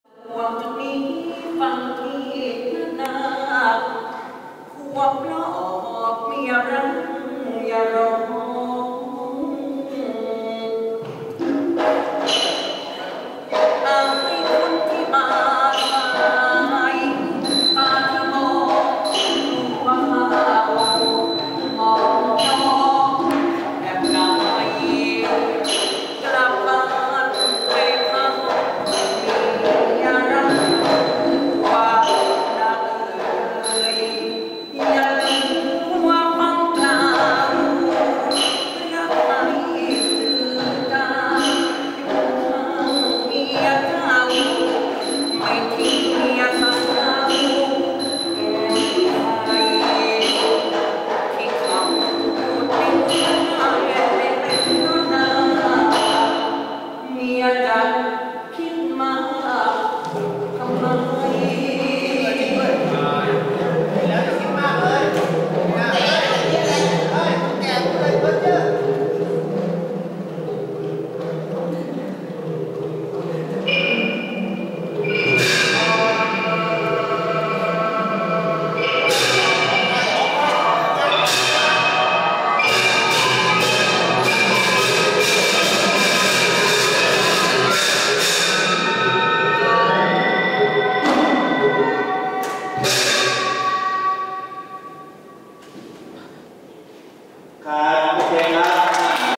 ลิเกนิรันดร์ อัญชลี เรื่อง"แม่นาคพระโขนง"(ฉากที่1 ลานวัด)27 พ.ค 58
27 พฤษภาคม 2558
ลิเกคณะ นิรันดร์ อัญชลี
บันทึกเทป เรื่อง "แม่นาคพระโขนง"
ณ บริษัท โรสมีเดีย